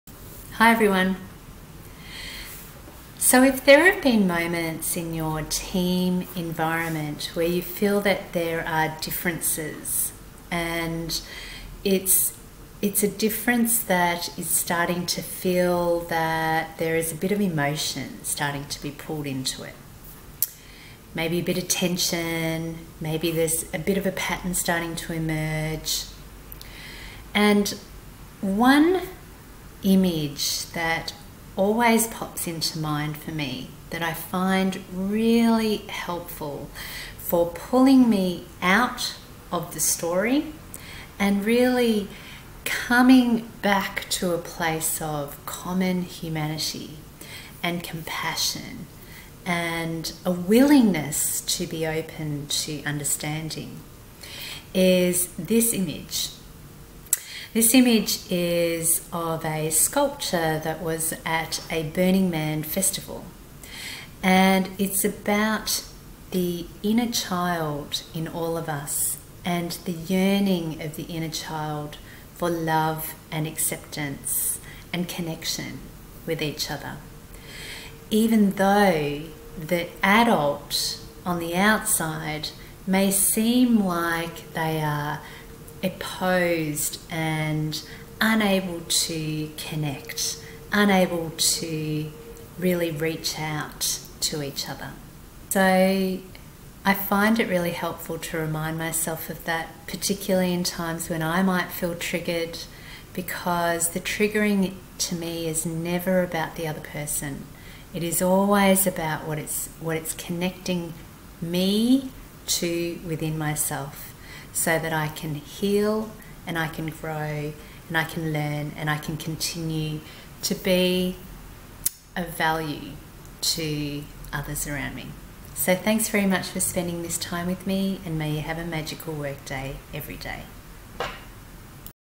What if your team isn't collaborating or there's tension?
Are there differences felt in the dynamics in your team environment where there might be a bit of tension or is there a pattern of behaviour emerging that might not be helpful to collaboration, performance  and wellbeing? This image brings me to a place of compassion and the connection. Even if what may appear on the outside might seem to be differences on the surface.

Love to hear your thoughts 🙏

May you have a magical workday, every day!